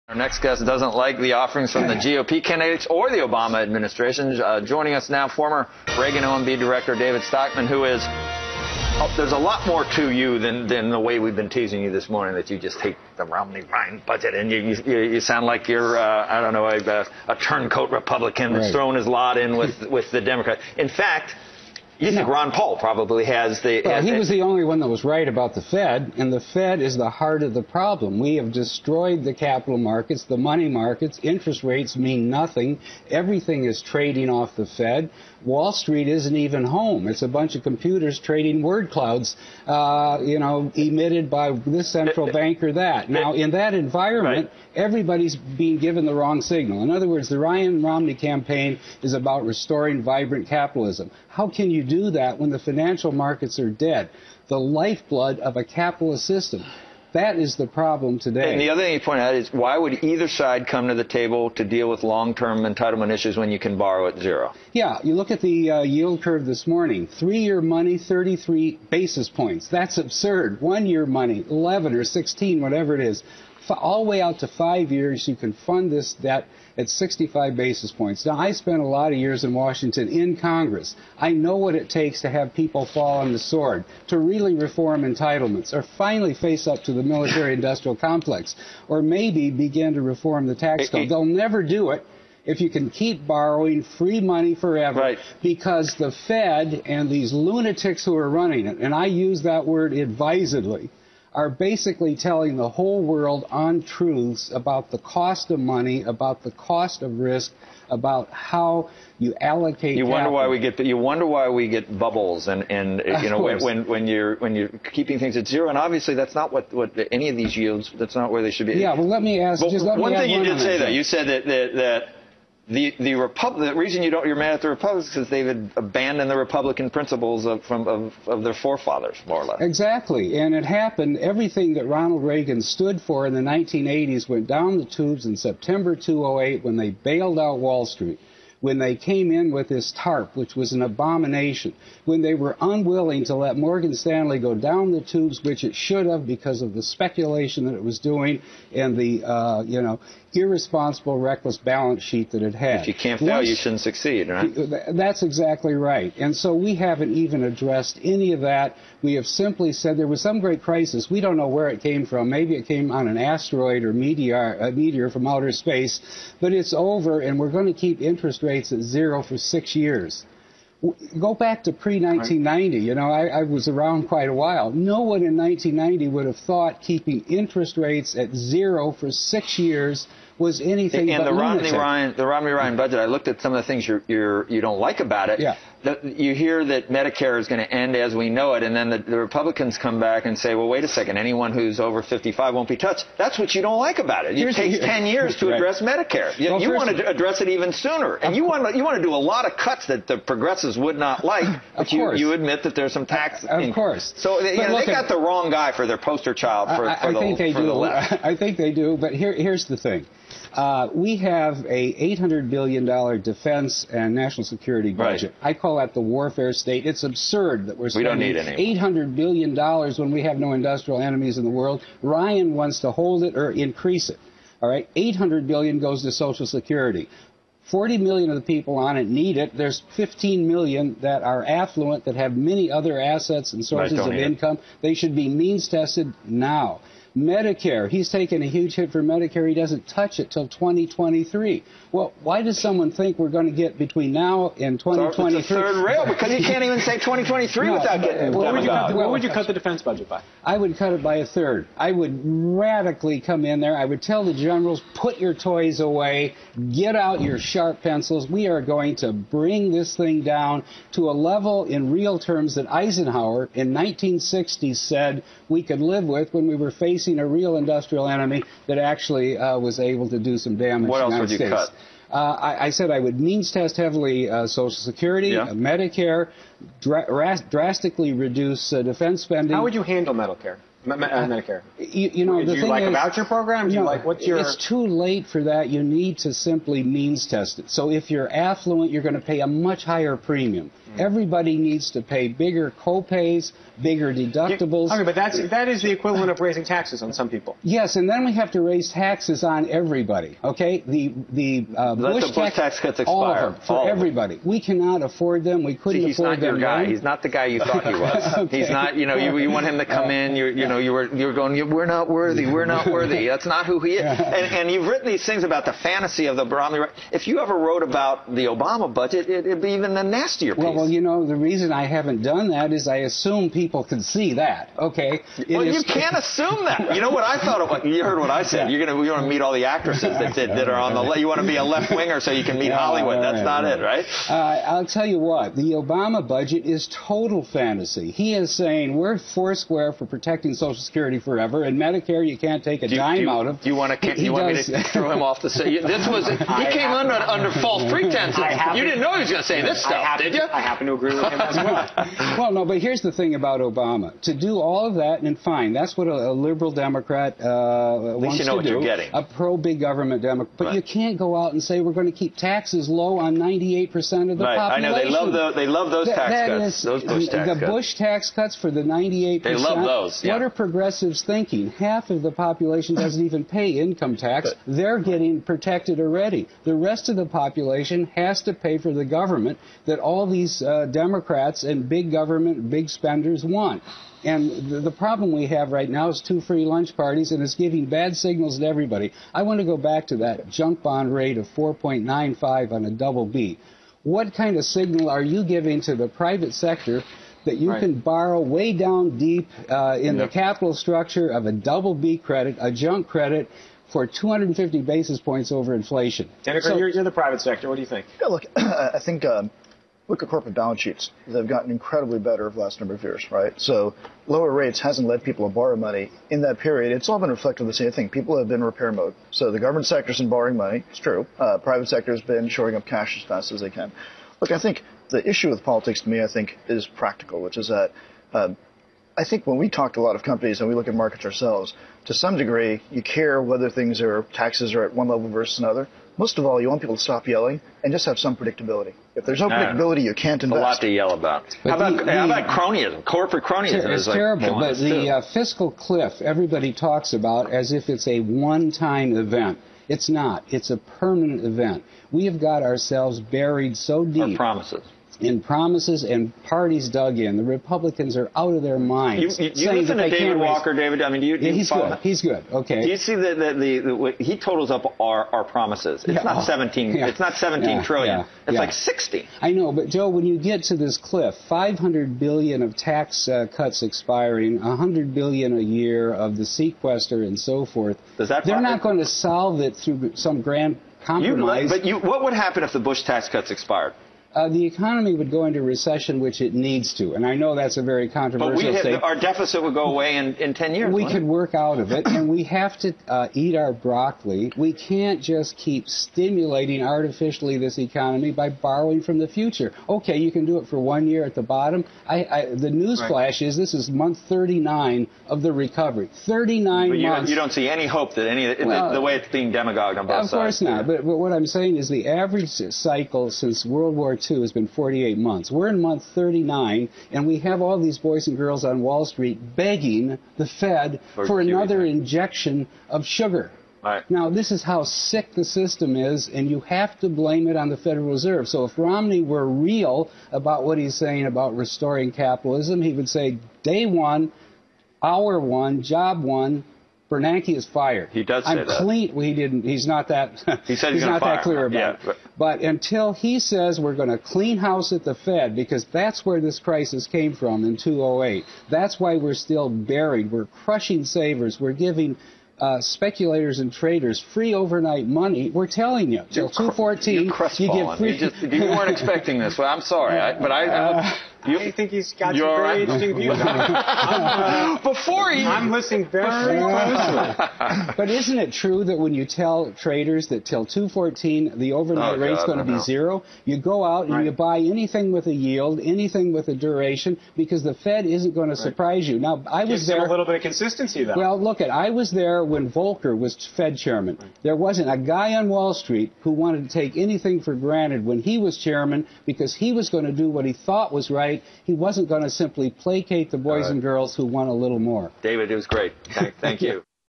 David Stockman told CNBC - FED is the problem - Ron Paul was Right!
Former OMB boss David Stockman told CNBC that Romney's vision of capitalism is not possible if the Federal Reserve continues to run the economy. Stockman said the only candidate that had it right was Ron Paul.

The Federal Reserve must be dismantled immediately and the banksters prosecuted for their criminal conspiracy to destroy America. Short of that, we can only expect to be not only fleeced, but as Jefferson warned, made homeless on the continent our fathers conquered.